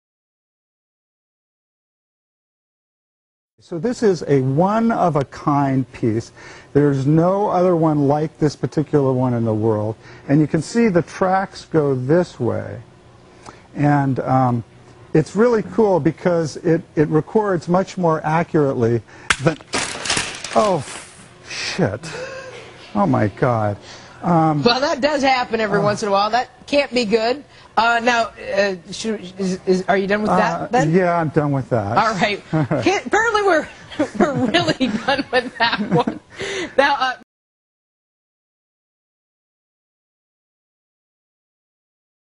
Guy Breaks Priceless Relic
From TechTV, guest breaks priceless phonograph tube.